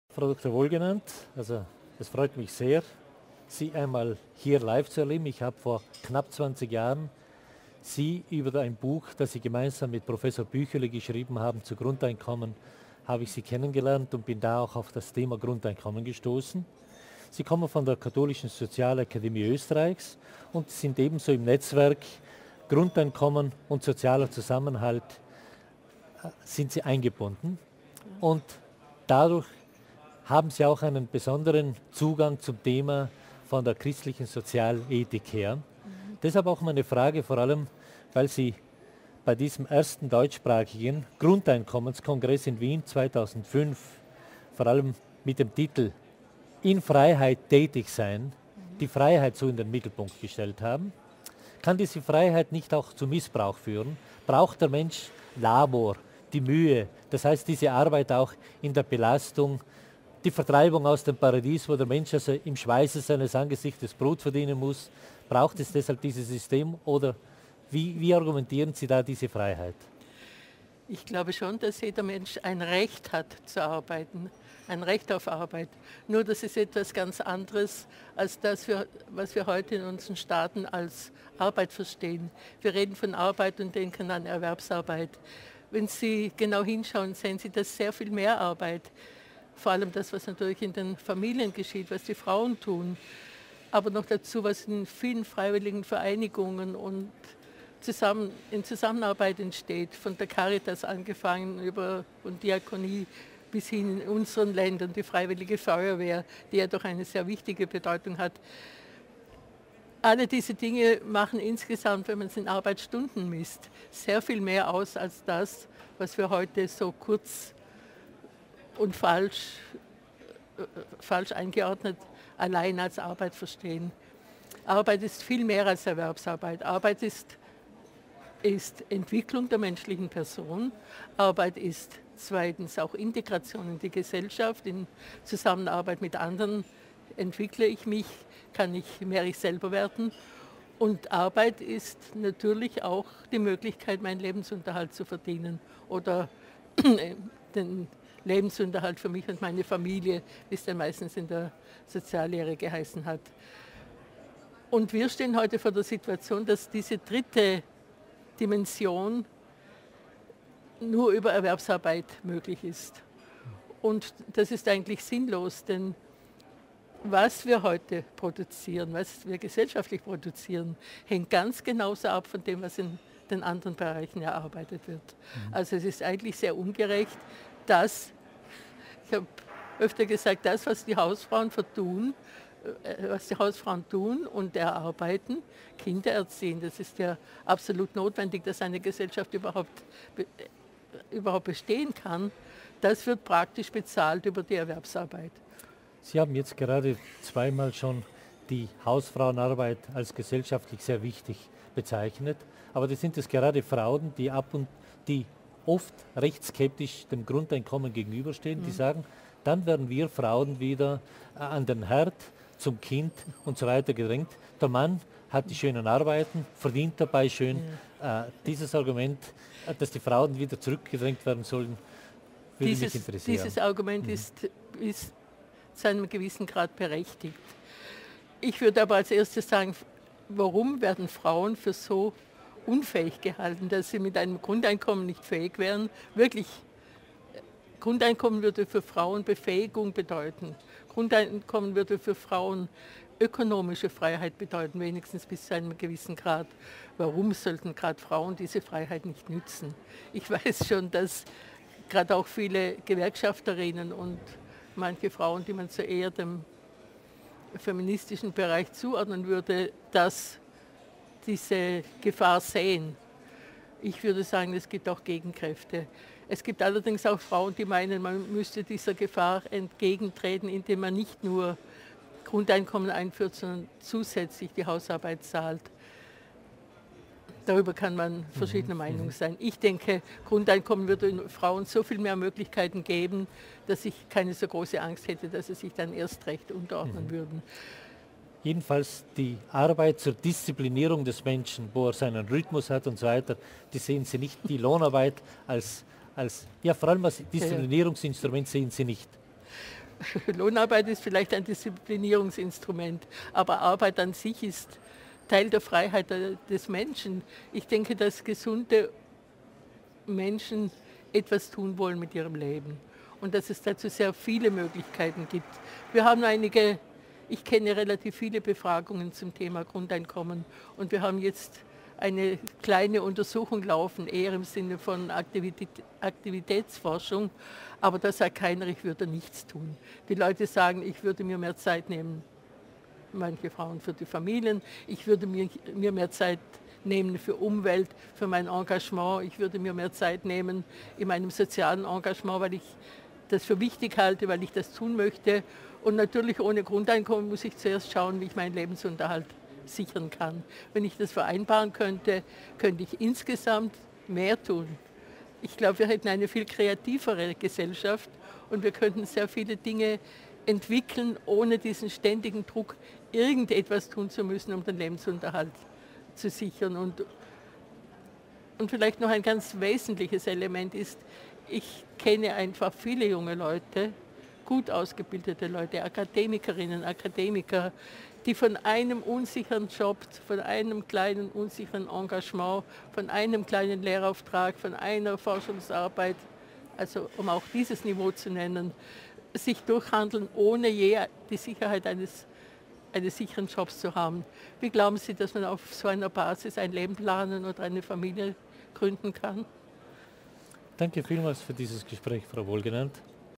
Sepp Kusstatscher and Lieselotte Wohlgenannt über Grundeinkommen
Der Mensch braucht Beschäftigung, nicht jedoch Lohnarbeit. Wäre das Leben schöner, wenn der Druck auf Leistung weggenommen würde?